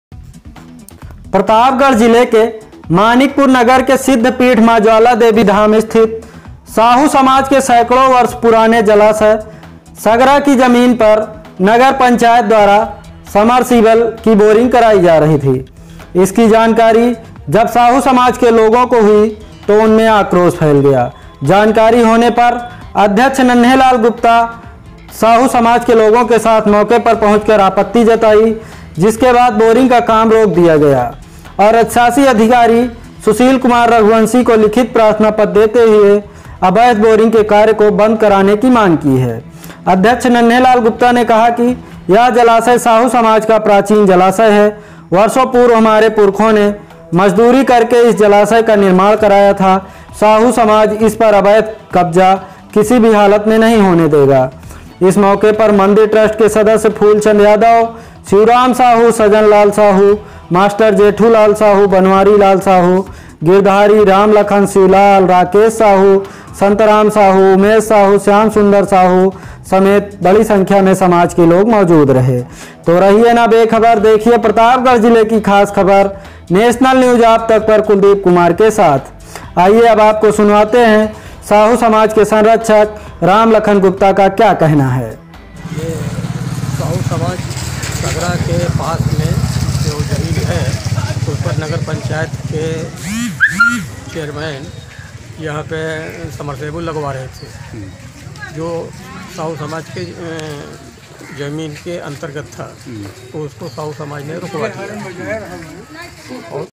प्राचीन सगरे की जमीन पर नगर पंचायत करवा रहा बोरिंग, साहू समाज में आक्रोश
प्रतापगढ़ जिले के मानिकपुर नगर के सिद्ध पीठ मां ज्वाला देवी धाम स्थित साहू समाज के सैकड़ों वर्ष पुराना जलाशय ( सगरा) की जमीन पर नगर पंचायत द्वारा समर्सिबल की बोरिंग कराई जा रही थी । इसकी जानकारी जब साहू समाज के लोगों को हुई उनमें आक्रोश फैल गया । जानकारी होने पर अध्यक्ष नन्हे लाल गुप्ता साहू समाज के लोगों के साथ मौके पर पहुंचकर आपत्ति जताई । जिसके बाद बोरिंग का काम रोक दिया गया और अधिशासी अधिकारी सुशील कुमार रघुवंशी को लिखित प्रार्थना पत्र देते हुए अवैध बोरिंग के कार्य को बंद करने की मांग की है।  अध्यक्ष नन्हेलाल गुप्ता ने कहा कि यह जलाशय साहू समाज का प्राचीन जलाशय हैं। वर्षो पूर्व हमारे पुरखों ने मजदूरी करके इस जलाशय का निर्माण कराया था। साहू समाज इस पर अवैध कब्जा किसी भी हालत में नहीं होने देगा । इस मौके पर मंदिर ट्रस्ट के सदस्य फूलचंद यादव ,शिवराम साहू ,सजन लाल साहू, मास्टर जेठू लाल साहू, बनवारी लाल साहू, गिरधारी, राम लखन, शिवलाल ,राकेश साहू , संतराम साहू, उमेश साहू, श्यामसुंदर साहू सहित भारी संख्या में साहू समाज के लोग मौजूद रहे।